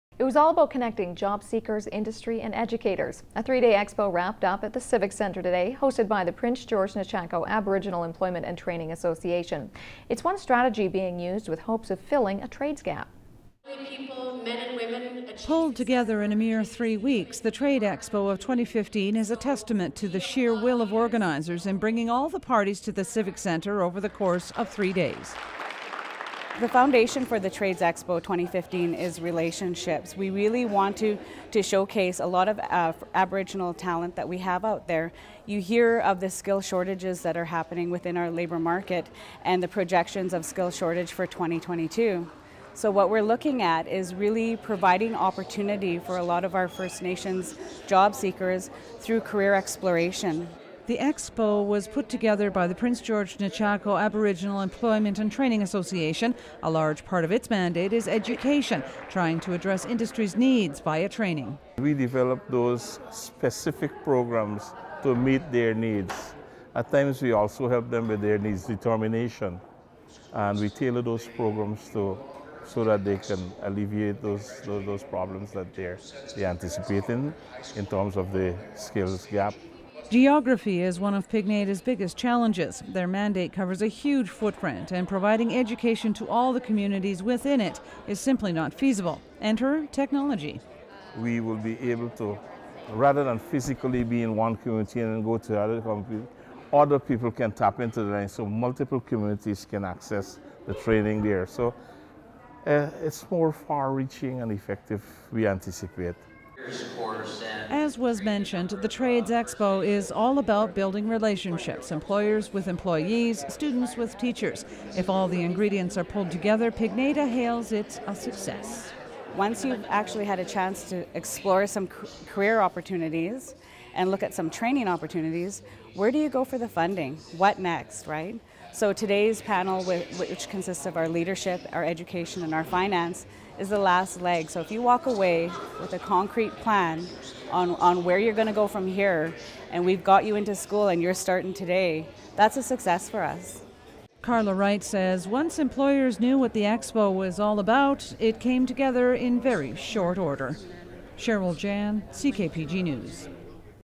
First Nations Consider Trades Jobs
It was all about connecting job seekers, industry and educators. A three day expo wrapped up at the Civic Centre today hosted by the Prince George Nechako Aboriginal Employment and Training Association. It's one strategy being used with hopes of filling a trades gap.